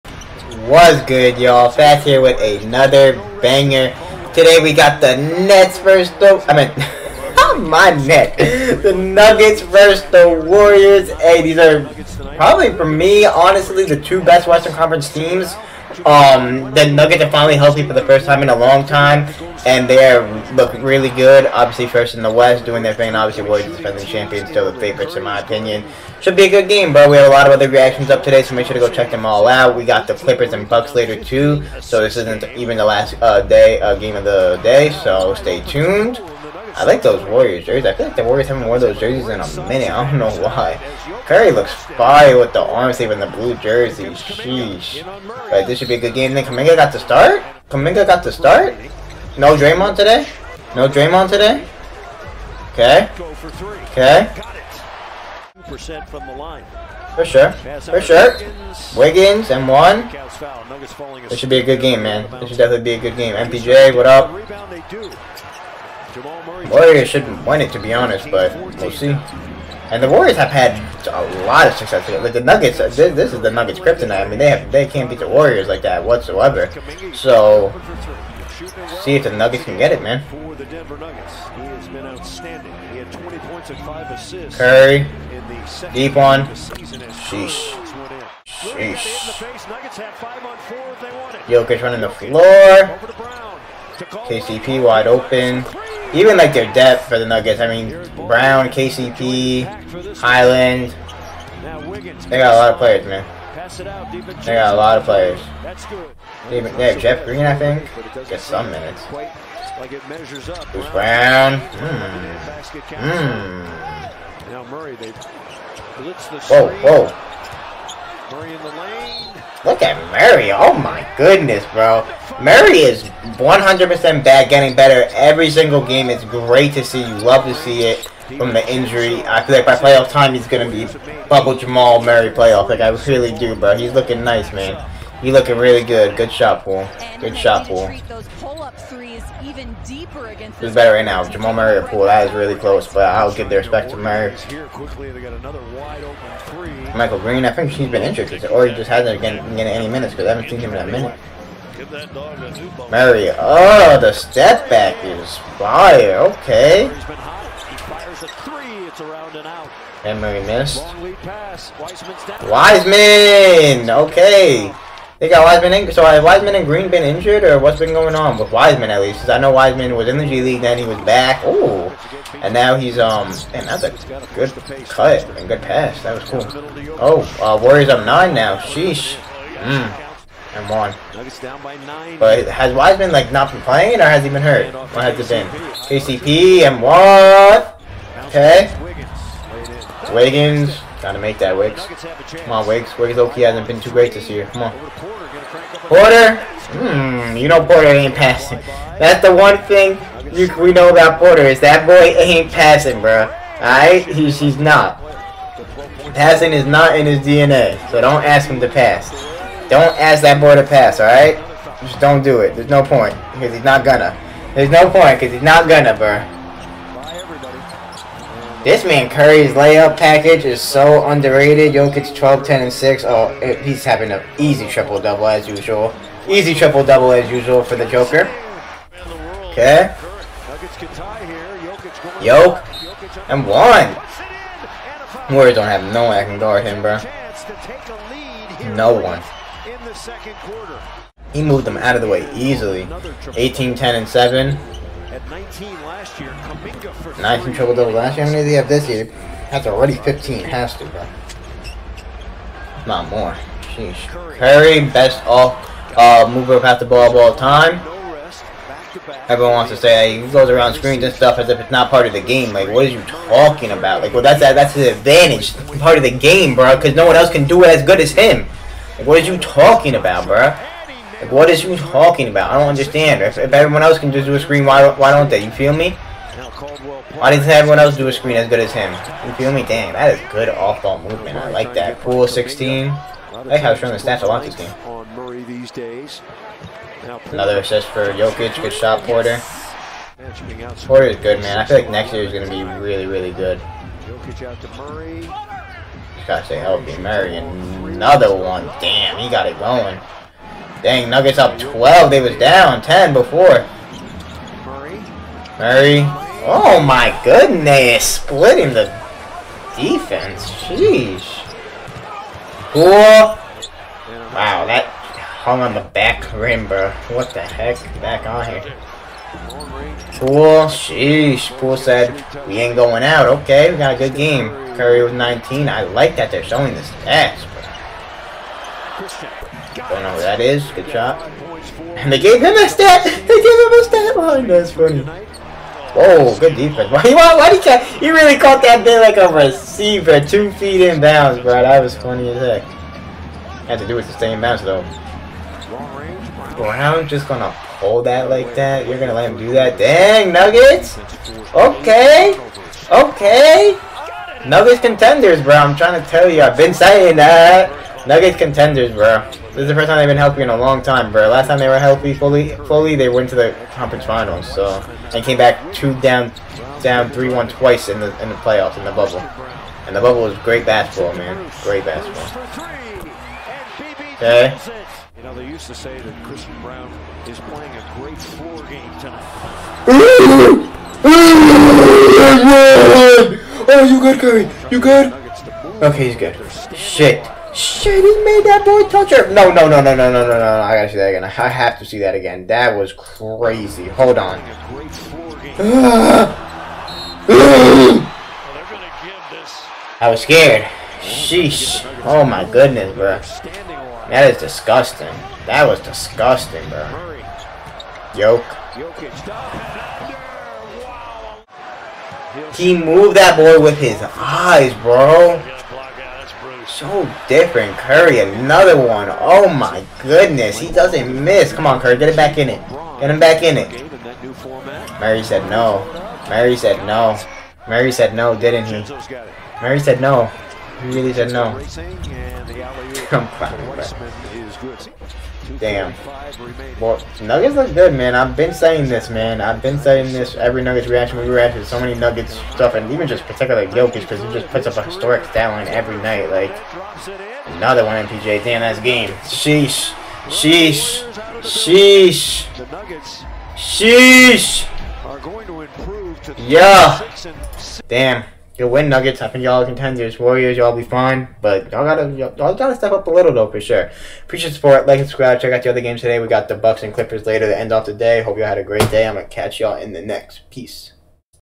NUGGETS CONTENDERS? GOLDEN STATE WARRIORS VS DENVER NUGGETS 2/2/23 FULL HIGHLIGHTS REACTION!
IN THIS VIDEO I REACTED TO WARRIORS VS NUGGETS! LET ME KNOW YOUR THOUGHTS!